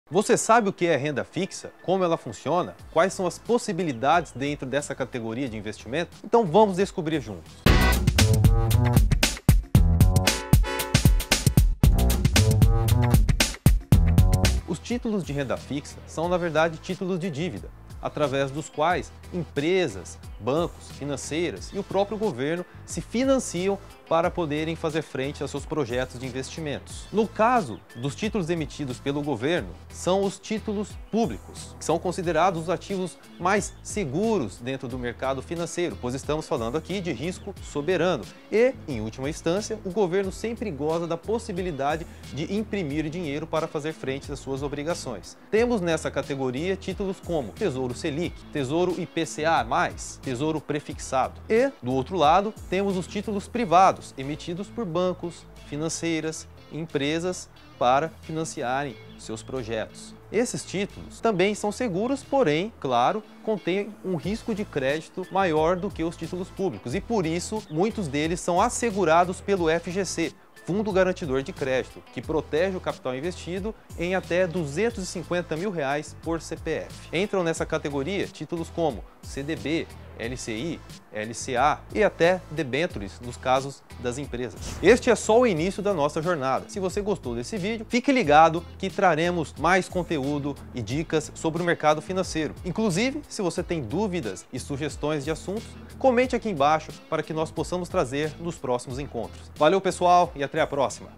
O que é RENDA FIXA?! Com Vitor Hugo (Mentor da ATOM)
O que é Renda Fixa?

No vídeo de hoje você vai entender o que são títulos públicos (Tesouro SELIC, Tesouro IPCA+ e Tesouro Pré-Fixado) e títulos privados (CDB, LCI, LCA e debêntures).

Gostou do conteúdo do nosso canal?